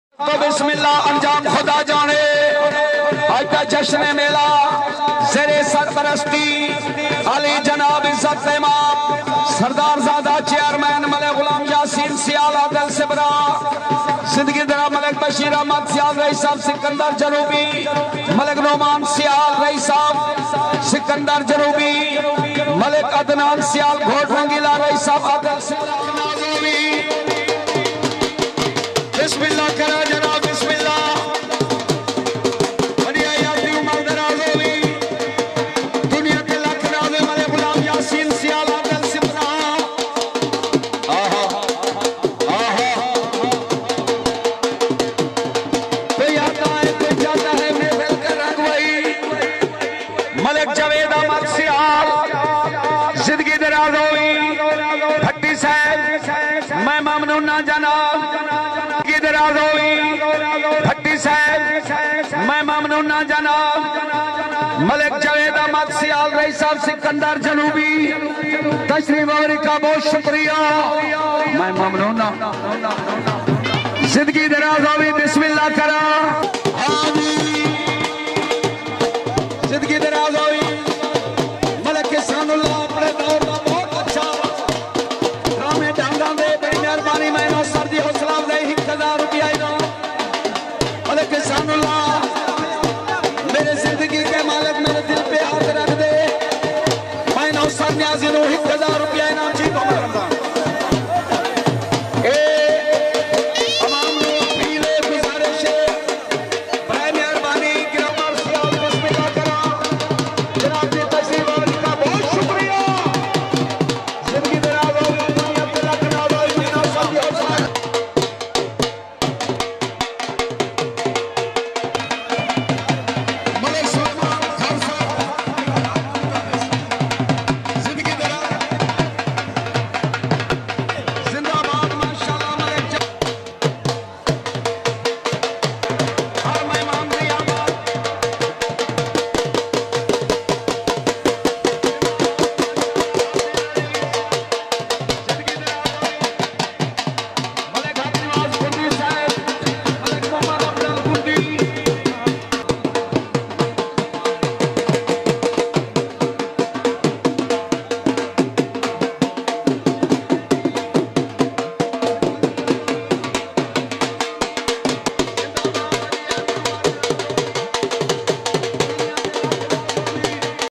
4 March 2023
شادی پروگرام عادل سپرا
دولہا ملک عدنان احمد سیال
والد عطاءاللّٰه سیال مرحوم
بھائی ملک عمر فاروق سیال
بانی میلہ چیئرمین سردار ملک غلام یاسین سیال